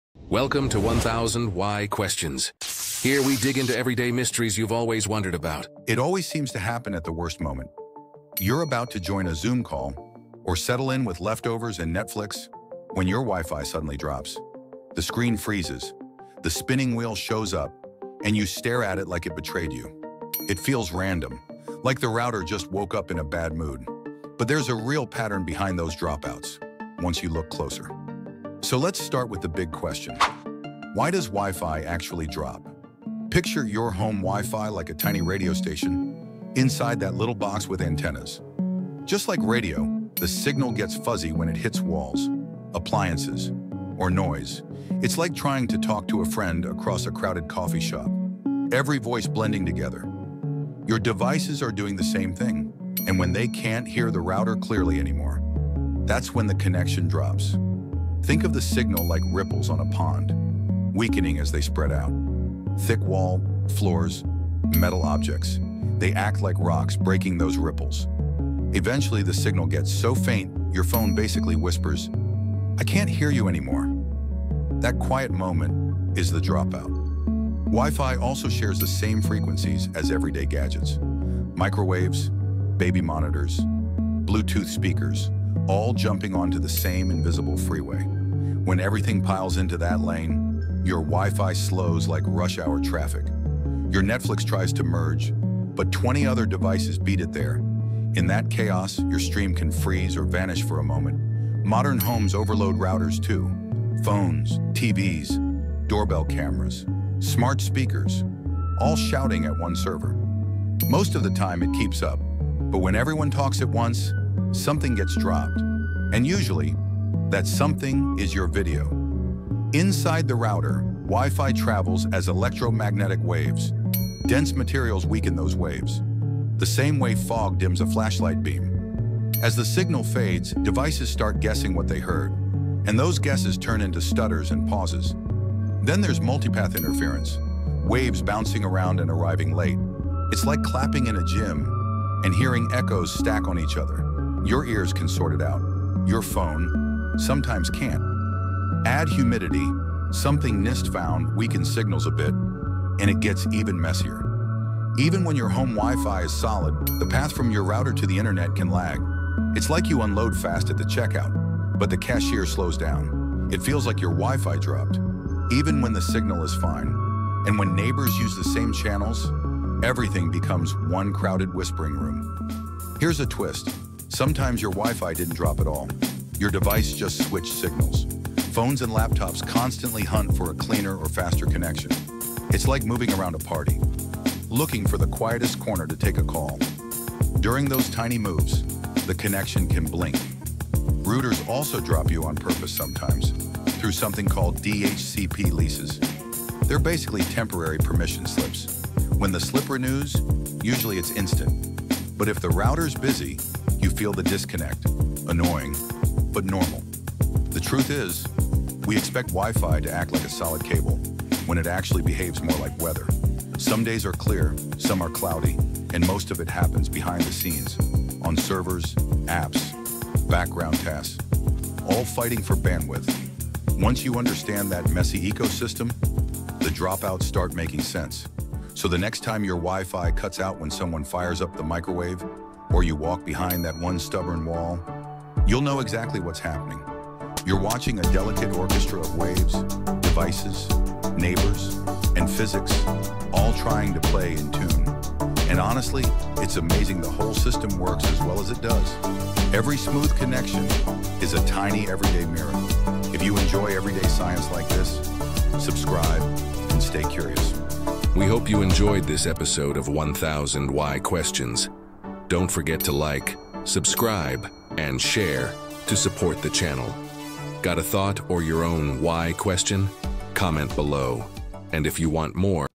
Why Your Wi-Fi Keeps Dropping (Even When Nothing Changed)
Why does your Wi-Fi suddenly drop, slow down, or freeze at the worst possible moment? In this video, we break down why Wi-Fi disconnects, the real science behind signal drops, and the everyday factors that quietly ruin your connection. Whether you're streaming, gaming, or working from home, understanding this helps you fix it faster.
Here’s what you’ll learn:
• The real reasons Wi-Fi drops: interference, congestion, distance, and hardware limits.
• Everyday disruptors you didn’t know affect Wi-Fi—microwaves, walls, neighbors, and even your router’s age.
• How to stabilize your connection with simple fixes anyone can do at home.